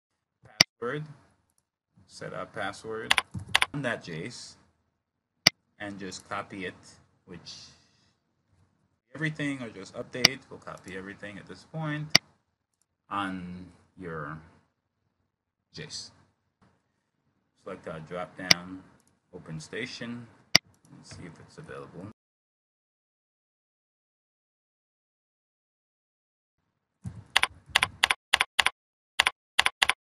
Niagara N4 Training - Create Copy Load New Station
Training on Niagara N4 BMS System - Create Copy Load New Station .  Create Copy Load New Station Training.  See link for Android training apps below.